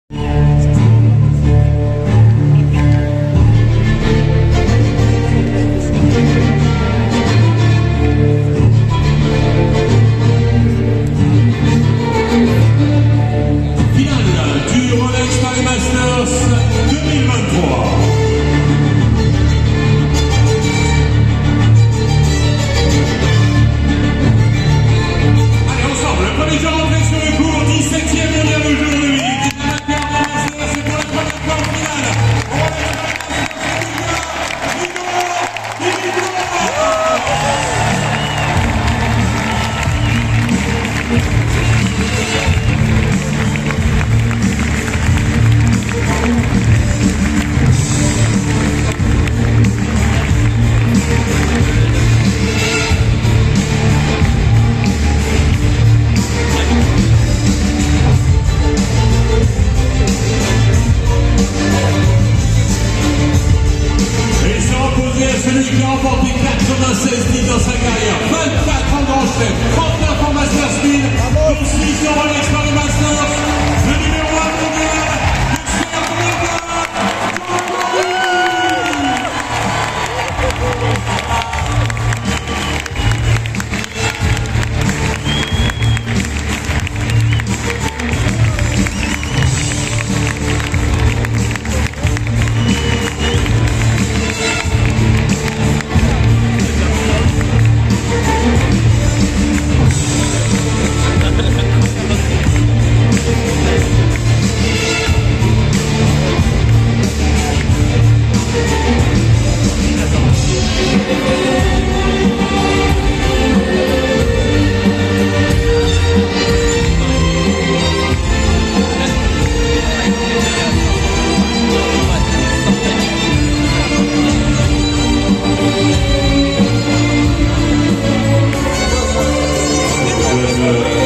🇷🇸Novak Djokovic vs. 🇧🇬Grigor Dimitrov Final ATP Masters Paris 2023🎾
Player presentation on final at @rolexparismasters Novak Djokovic vs. Grigor Dimitrov